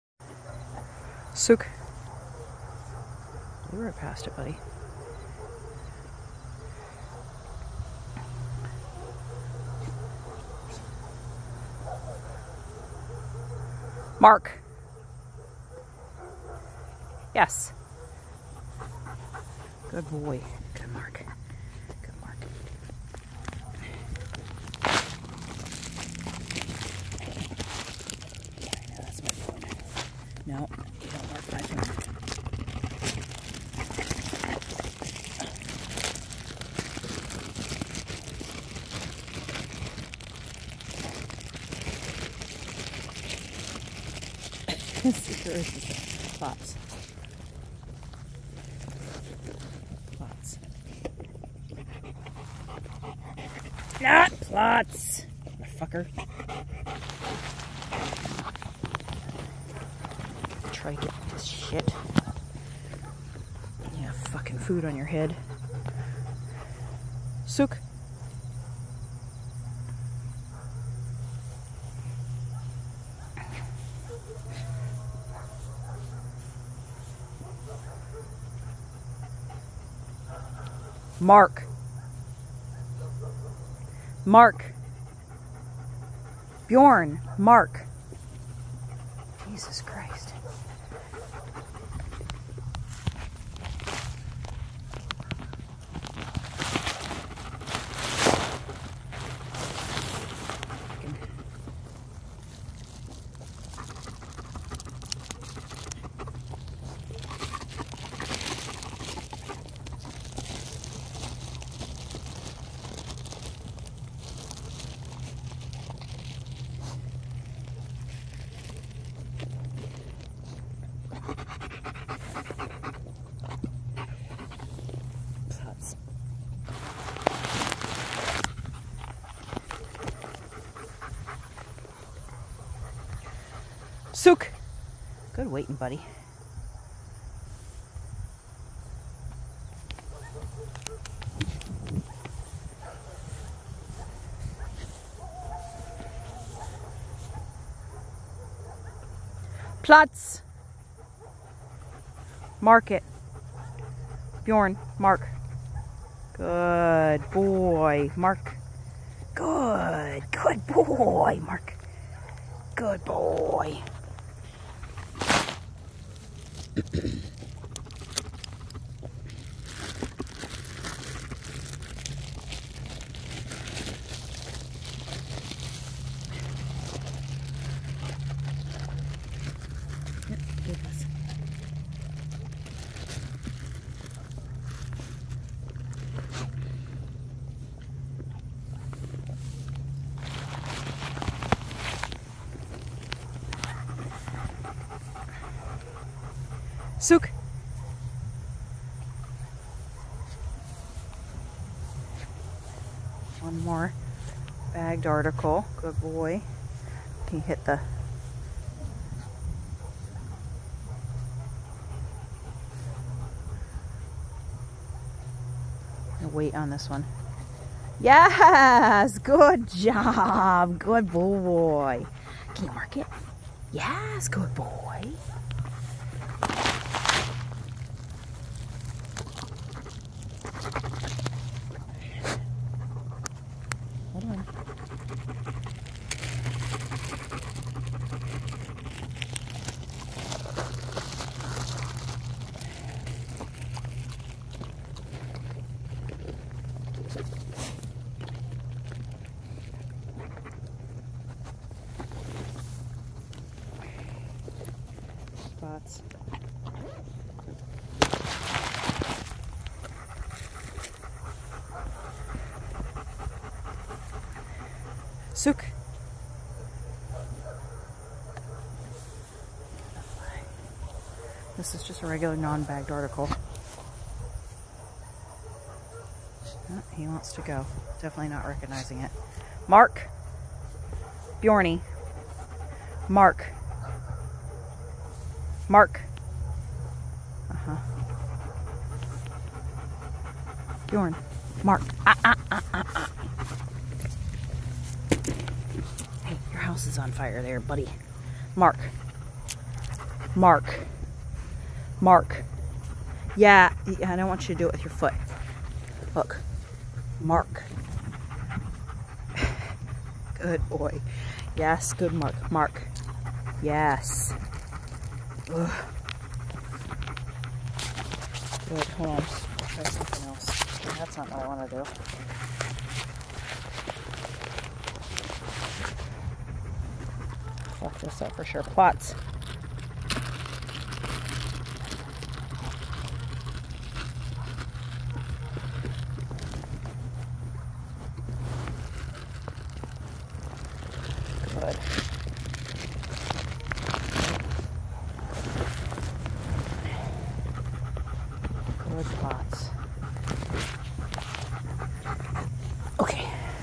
Bjorne Day 5 IGP tracking for meal
Shotty camera work while trying to handle and video simultaneously